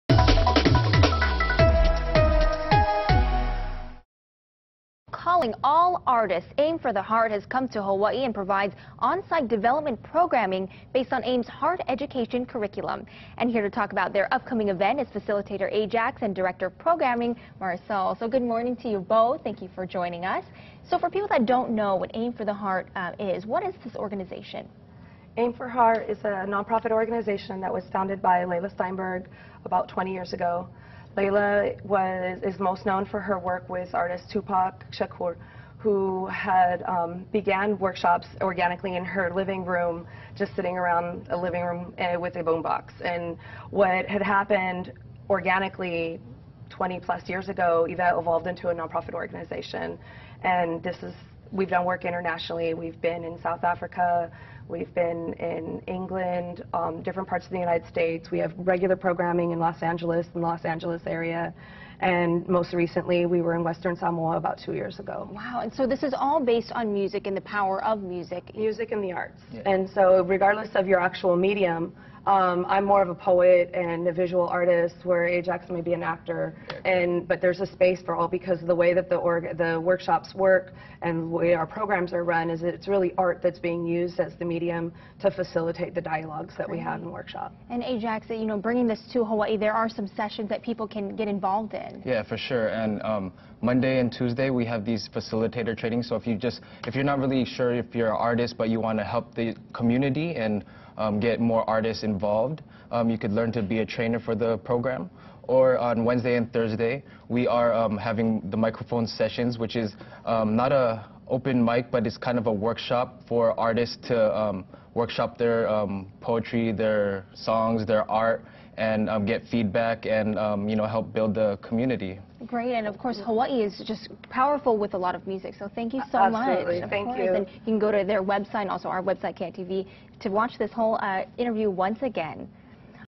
How you can AIM 4 the HeArt
Calling all artists! AIM for 4 the HeArt has come to Hawaii and provides on site development programming based on AIM's heART education curriculum. Find out how you can get involved! For news you need to know,